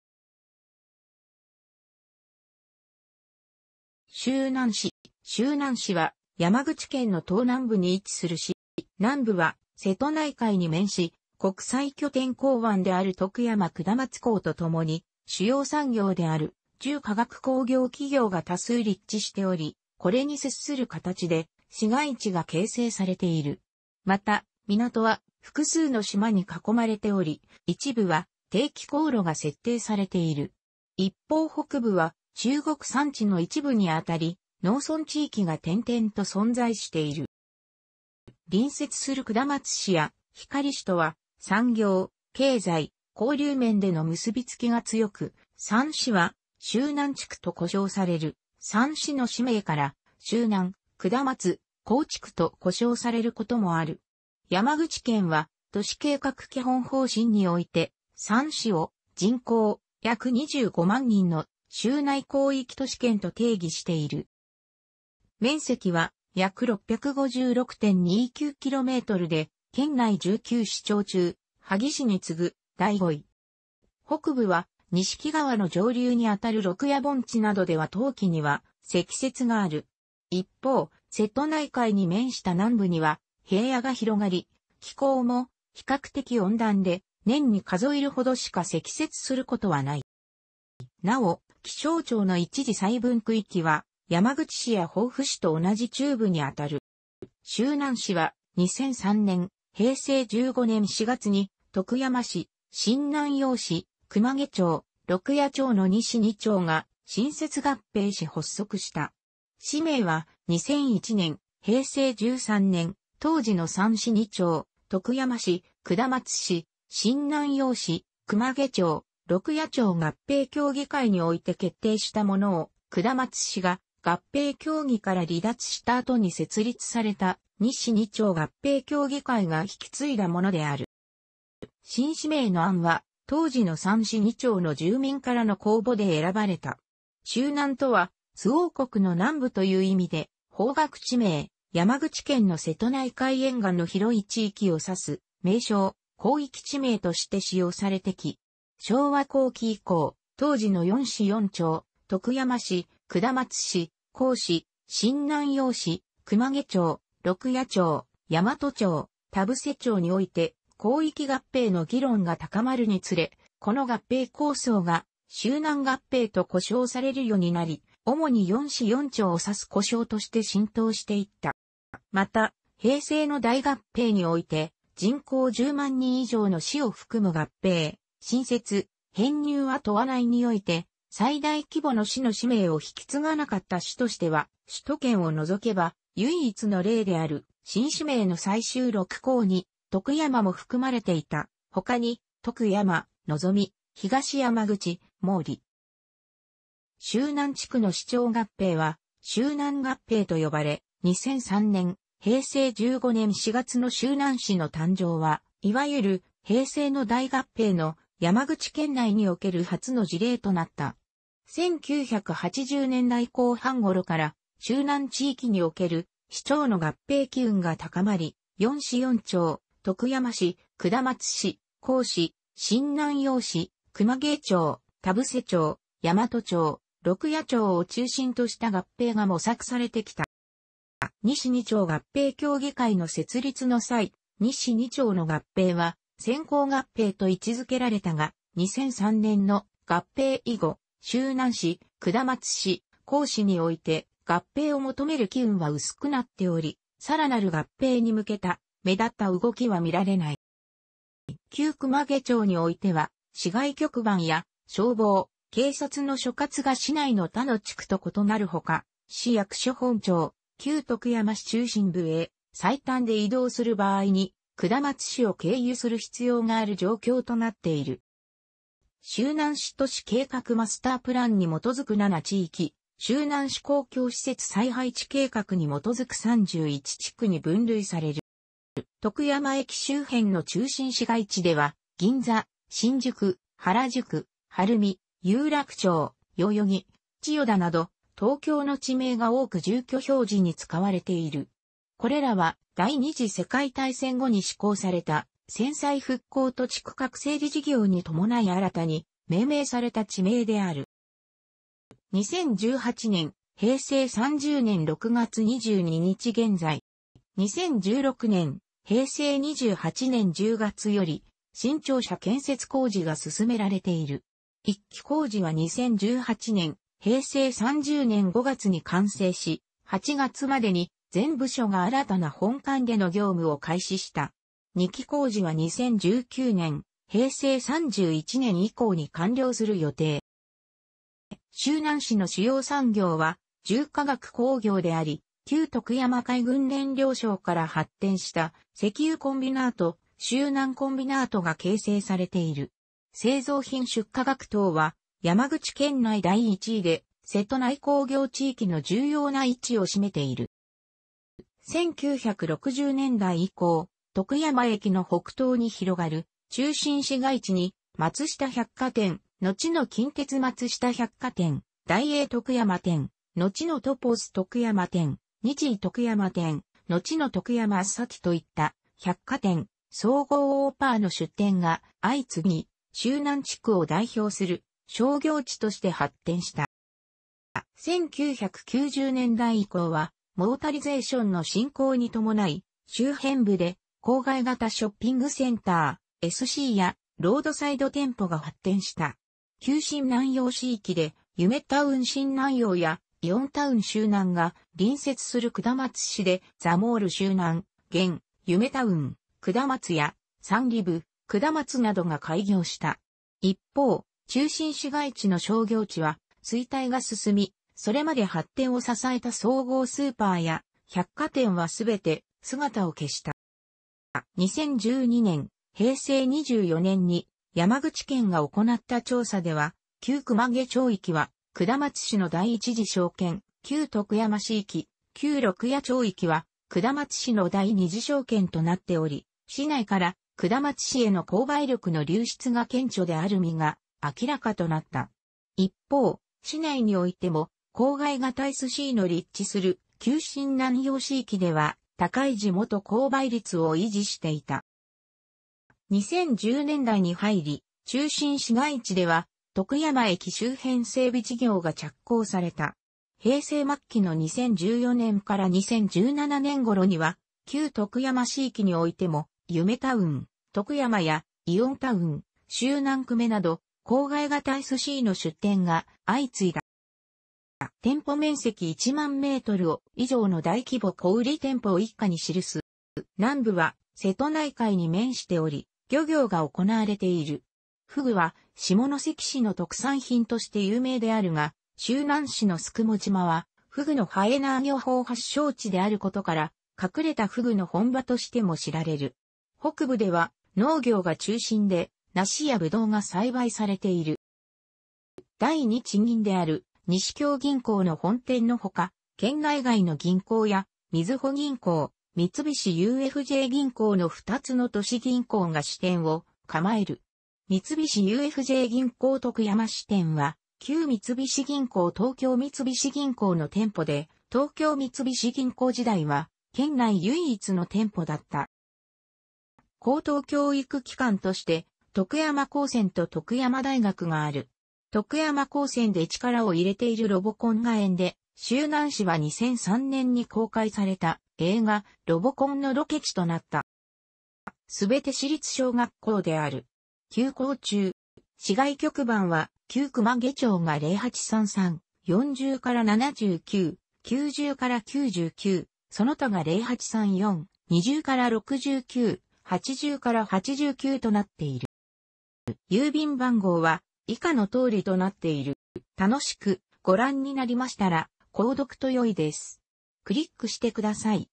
周南市
周南市（しゅうなんし）は、山口県の東南部に位置する市。

南部は瀬戸内海に面し、国際拠点港湾である徳山下松港とともに、主要産業である重化学工業企業が多数立地しており、これに接する形で市街地が形成されている。また、港は複数の島に囲まれており、一部は定期航路が設定されている。一方北部は中国山地の一部にあたり、農村地域が点々と存在している。

隣接する下松市や光市とは、産業・経済・交流面での結びつきが強く、3市は「周南地区」と呼称される（3市の市名から「周南・下松・光地区」と呼称されることもある）。山口県は、都市計画基本方針において、3市を人口約25万人の「周南広域都市圏」と定義している。

面積は約656.29kmで、県内19市町中、萩市に次ぐ第5位。

北部は、錦川の上流にあたる鹿野盆地などでは冬季には積雪がある。一方、瀬戸内海に面した南部には平野が広がり、気候も比較的温暖で、年に数えるほどしか積雪することはない。

なお、気象庁の一次細分区域は、山口市や防府市と同じ中部にあたる

周南市は、2003年（平成15年）4月に徳山市、新南陽市、熊毛町、鹿野町の2市2町が新設合併し発足した。市名は、2001年（平成13年）、当時の3市2町（徳山市、下松市、新南陽市、熊毛町、鹿野町）合併協議会において決定したものを、下松市が合併協議から離脱した後に設立された、2市2町合併協議会が引き継いだものである。

新市名の案は、当時の3市2町の住民からの公募で選ばれた。「周南」とは周防国の南部という意味で（方角地名）、山口県の瀬戸内海沿岸の広い地域を指す名称（広域地名）として使用されてきた。昭和後期以降、当時の4市4町（徳山市、下松市、光市、新南陽市、熊毛町、鹿野町、大和町、田布施町）において広域合併の議論が高まるにつれ、この合併構想が「周南合併」と呼称されるようになり、主に4市4町を指す呼称として浸透していった。

また、平成の大合併において人口10万人以上の市を含む合併(新設・編入は問わない)において最大規模の市の市名を引き継がなかった市としては首都圏を除けば唯一の例である（新市名の最終6候補に徳山も含まれていた（他に、とくやま、のぞみ、東山口、毛利）。）。

周南地区の市町合併は周南合併と呼ばれ、 2003年（平成15年）4月の周南市の誕生は、いわゆる「平成の大合併」の、山口県内における初の事例となった。1980年代後半ごろから周南地域における市町の合併機運が高まり、4市4町（徳山市、下松市、光市、新南陽市、熊毛町、田布施町、大和町、鹿野町）を中心とした合併が模索されてきた。

2市2町合併協議会の設立の際、2市2町の合併は「先行合併」と位置付けられたが、2003年の合併以後、周南市、下松市、光市において合併を求める機運は薄くなっており、更なる合併に向けた目立った動きはみられない。

旧熊毛町においては、市外局番や、消防・警察の所轄が市内の他の地区と異なるほか、市役所本庁（旧徳山市中心部）へ最短で移動する場合に下松市を経由する必要がある状況となっている。

周南市都市計画マスタープランに基づく7地域、周南市公共施設再配置計画に基づく31地区に分類される。

徳山駅周辺の中心市街地では、銀座、新宿、原宿、晴海、有楽町、代々木、千代田など、東京の地名が多く住居表示に使われている。これらは第二次世界大戦後に施工された戦災復興土地区画整理事業に伴い新たに命名された地名である。

2018年（平成30年）6月22日現在。

2016年（平成28年）10月より、新庁舎建設工事が進められている。1期工事は2018年（平成30年）5月に完成し、8月までに全部署が新たな本館での業務を開始した。2期工事は2019年（平成31年）以降に完了する予定。

周南市の主要産業は重化学工業であり、旧徳山海軍燃料廠から発展した石油コンビナート（周南コンビナート）が形成されている。製造品出荷額等は山口県内第1位で、瀬戸内工業地域の重要な位置を占めている。

1960年代以降、徳山駅の北東に広がる中心市街地に、松下百貨店（のちの近鉄松下百貨店）、ダイエー徳山店（のちのトポス徳山店）、ニチイ徳山店（のちの徳山サティ）といった百貨店・総合スーパーの出店が相次ぎ、周南地区を代表する商業地として発展した。

1990年代以降は、モータリゼーションの進行に伴い、周辺部で郊外型ショッピングセンター（SC）やロードサイド店舗が発展した。旧新南陽市域でゆめタウン...